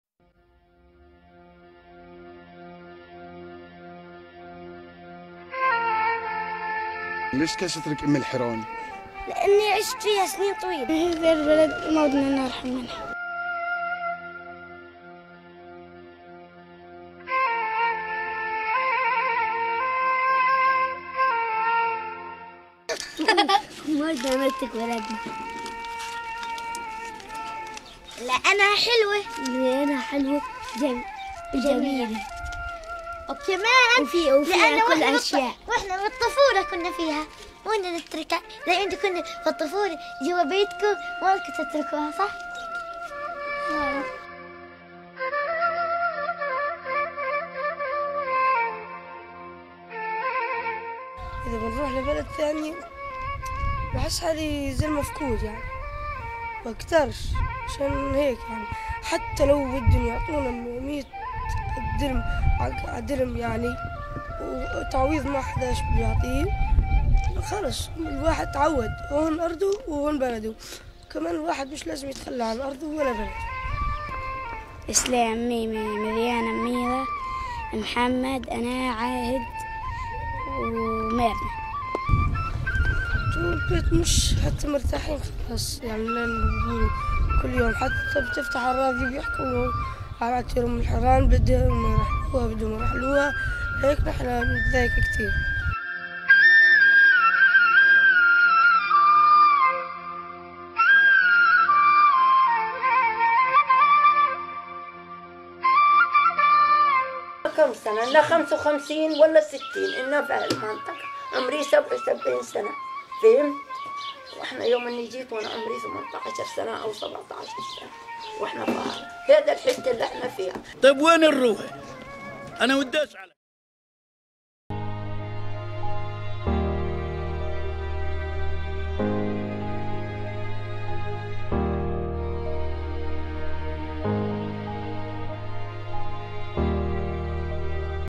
The Children of Umm al-Hiran respond to their village's immanent demolition
Testimonies from the kids of Umm al-Hiran, a Palestinian Bedouin village in the Negev. On 5 May, the Israeli Supreme Court decided in favour of the state to evict all 700 of the village's residents so that an Israeli Jewish community can be settled in their place. Join us as we advocate, litigate, mobilize and campaign to Save_UmAlHiran
Produced by George Batrice, Arrangement by Mostafa Qablawi

This video was shot during a 'social media storm' tour Adalah held for Arab journalists in Umm al-Hiran in late May 2015.